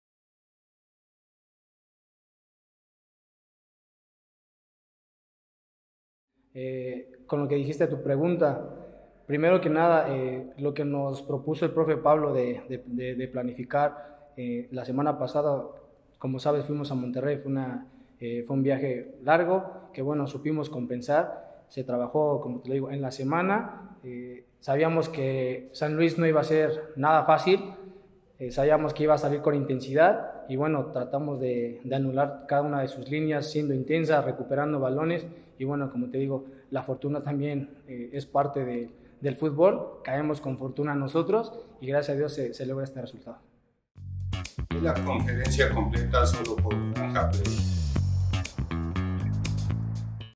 Conferencia de Prensa | Llegamos con la decisión de meter goles. La fortuna nos sonrió🎙🎽
El Profe Gabriel Hernández habló a los medios luego de la goleada de las Enfranjadas en el Cuauhtémoc. Escucha su sentir con la primera victoria en el AP22